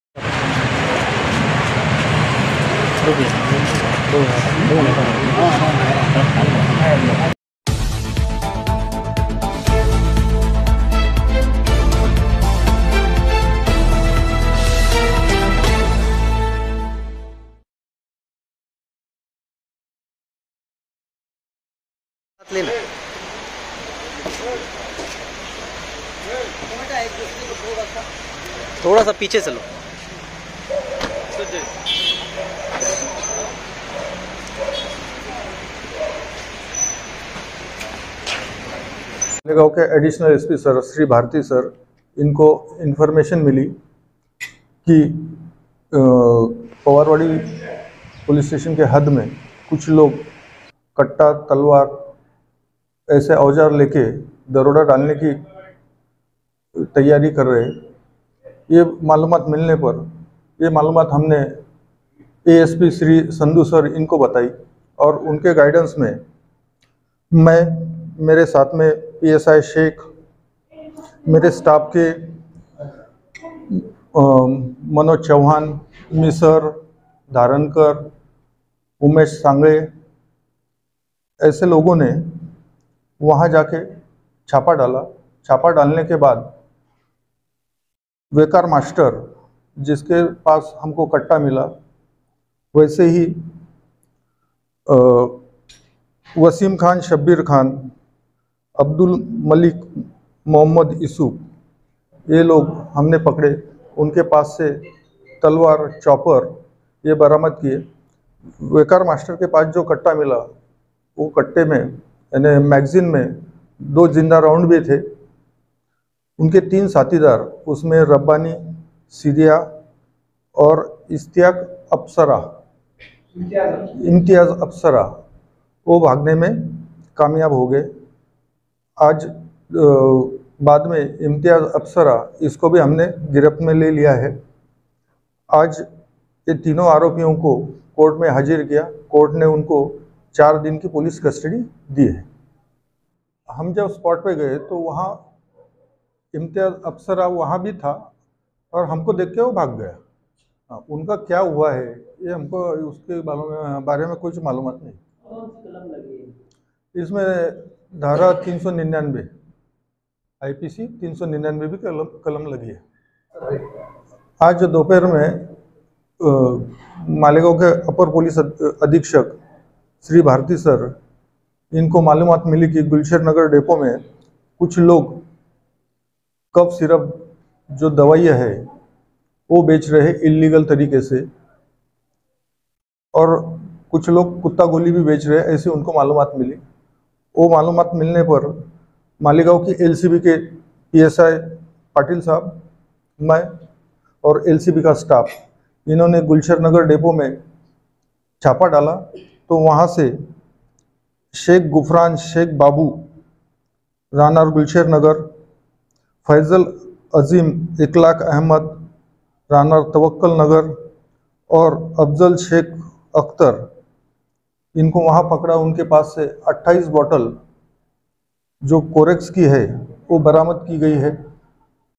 मालेगांव पवार वाड़ी पोलीस स्टेशन की 3 बड़ी कार्रवाई तलवार बंदूक कारतूस किए जप्त / SP / DYSP
بریکنگ نیوز
مالیگاؤں پواروڑی پولس اسٹیشن کی 2 بڑی کارروائی

وقار ماسٹر سمیٹ کئی افراد گرفتار
پستول کارتوس تلوار چاپڑ کیا ضبط

मालेगांव पवार वाड़ी पोलीस स्टेशन की दो बड़ी कार्रवाई